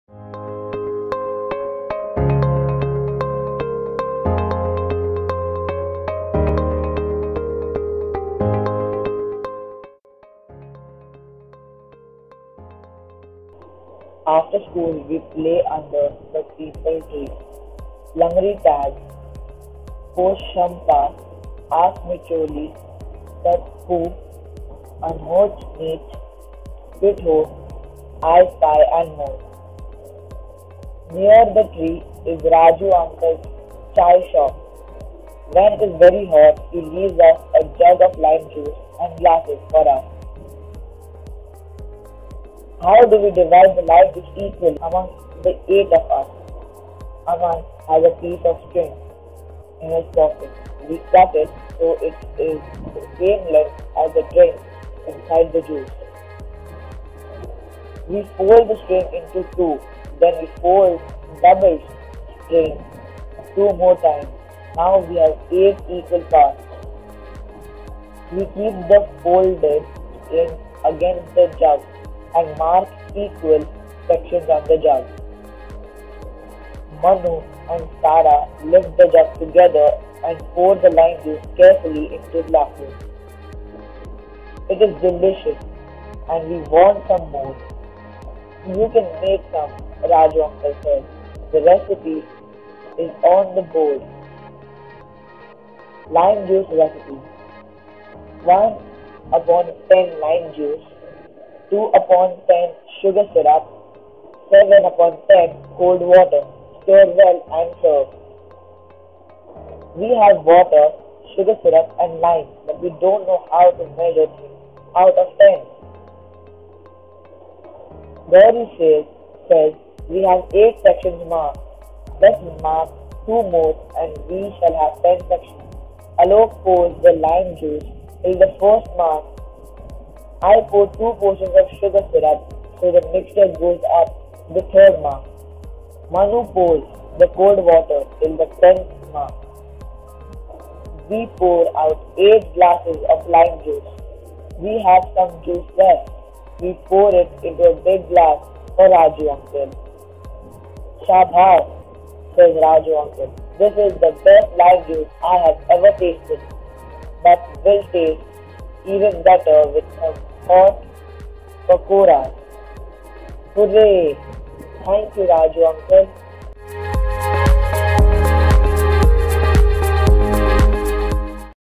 A Story A Day | Let's Make Lime Juice | Sahil
Let's learn fractions ➗  through today's story. Eight children try to make lime 🍋 juice from a recipe provided by uncle Raju and try to divide it equally. Are they successful? Did the lime juice taste the same?

'Let's Make Some Lime Juice!' is written by Nandini Bajpai, illustrated by Niloufer Wadia and published by Pratham Books. Sahil, the reader, enjoys reading books and believes that behaviour and not money shows the richness of an individual.